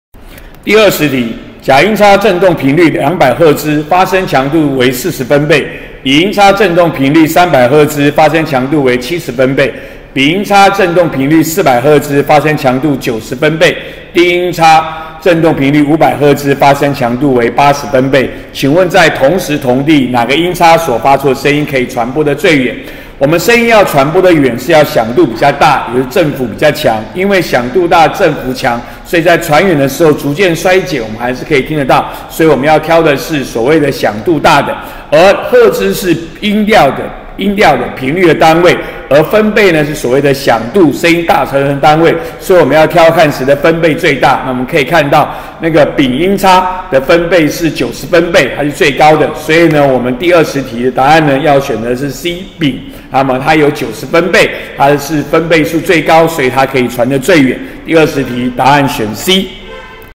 103學年上8年級理化二段第20題
103學年上學期八年級理化第二次段考第20題。給四音叉的頻率及發聲分貝，問何者可以傳得最遠。